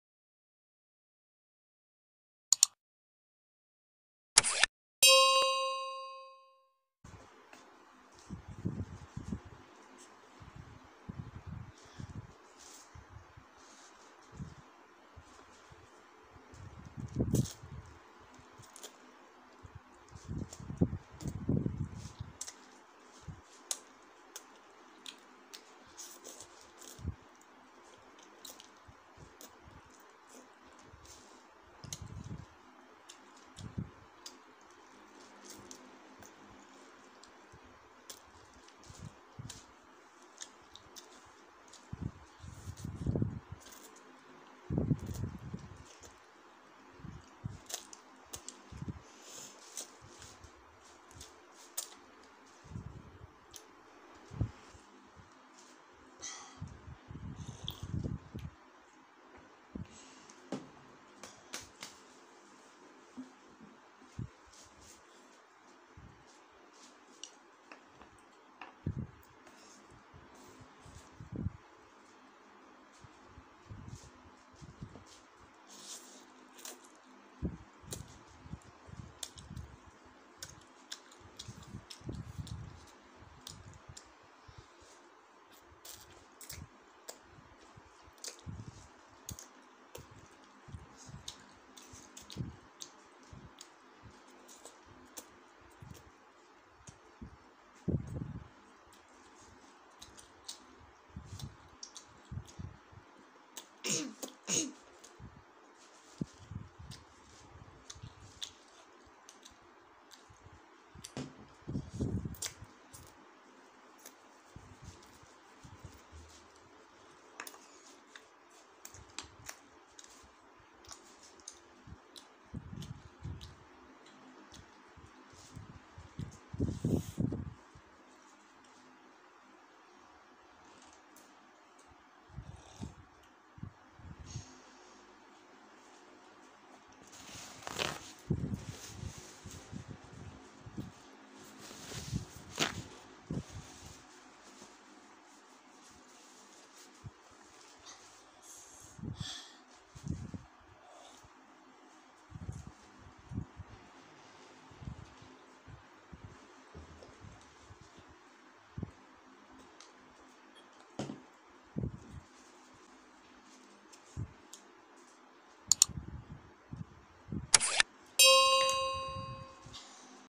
ramen cabonara (samyang) Malaysia
this video to short because i can't finish the ramen because to hot to me.